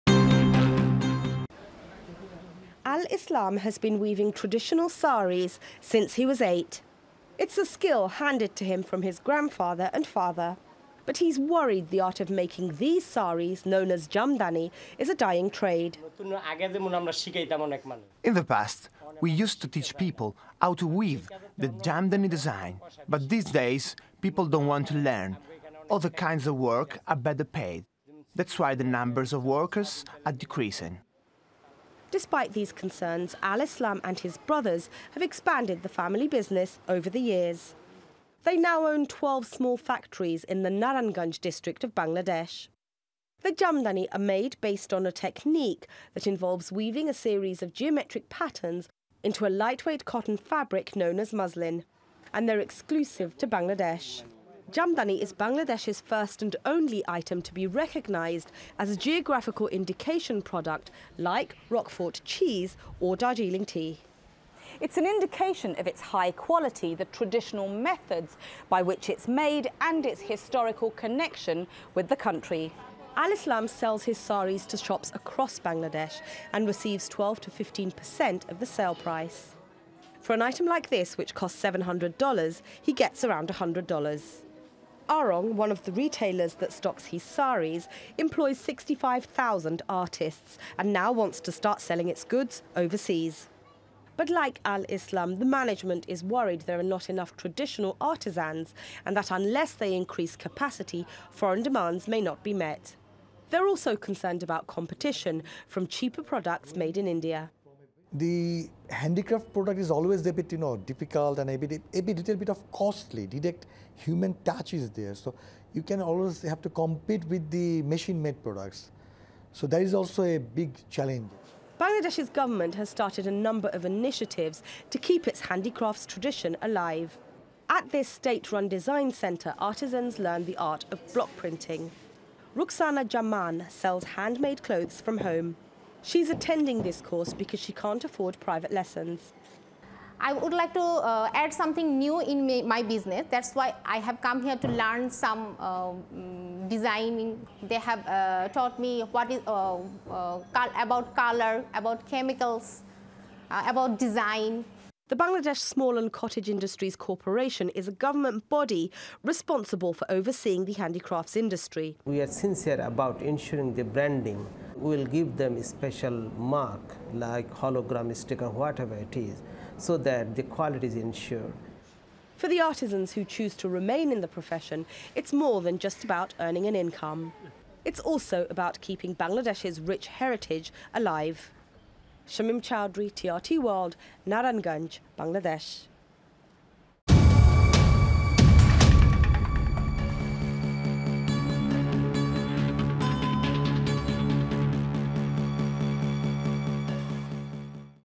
Bangladesh Booming: Traditional sari-weaving industry dying out
Bangladesh has a tradition of handicrafts and textile design that dates back hundreds of years. Many goods are produced by family-run businesses and then sold to retailers. The products are aimed mainly at a domestic market, but as Shamim Chowdhury discovers - these artisans now have grander plans.